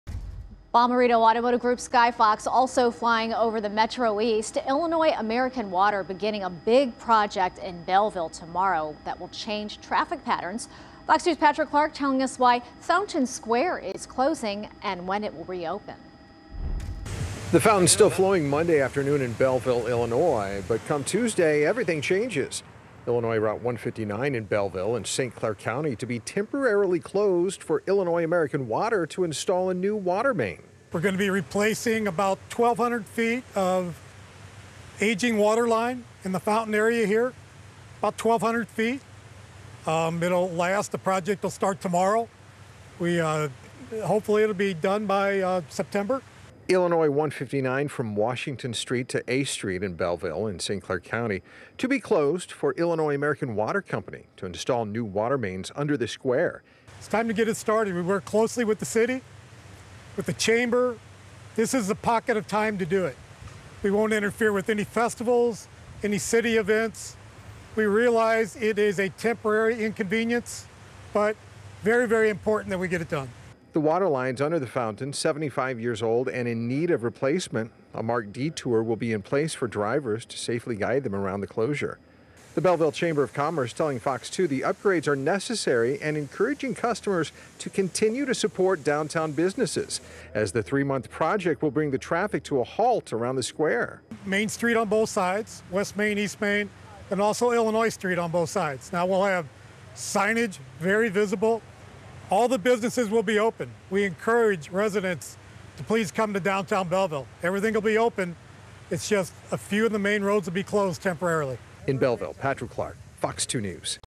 Belleville fountain shutting off for water main installation
The popular Veterans Memorial Fountain in Belleville Public Square will be turned off on Tuesday as Illinois American Water begins work to install a new water main.